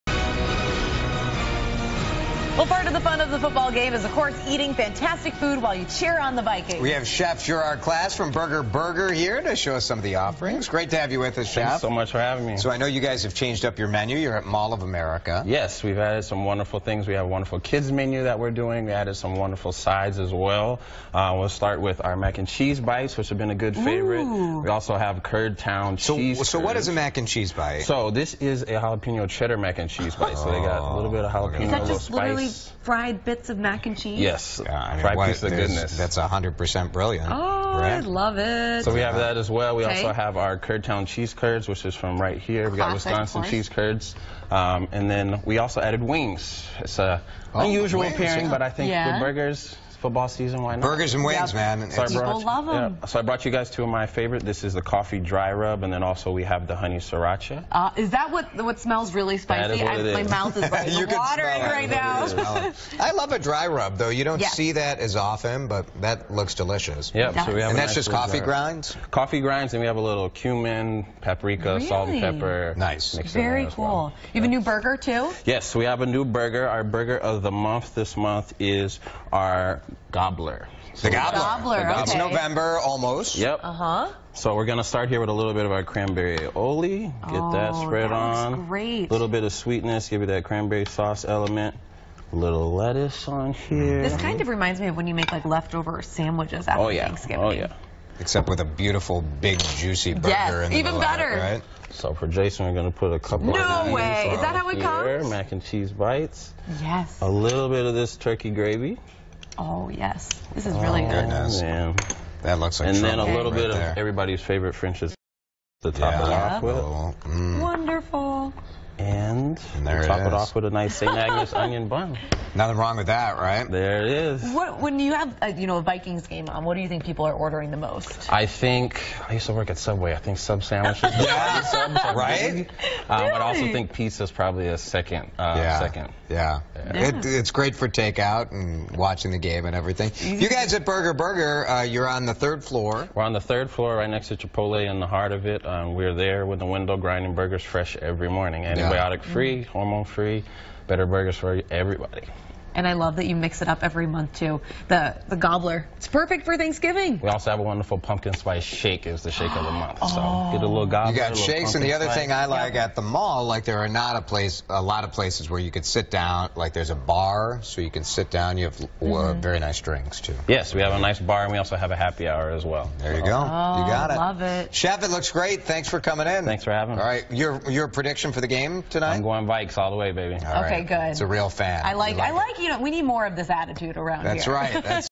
November Burger Of The Month - The Gobbler
Thank you to Kare 11 for having Executive Chef Gerard Klass on to demonstrate our November Burger of the Month - The Gobbler!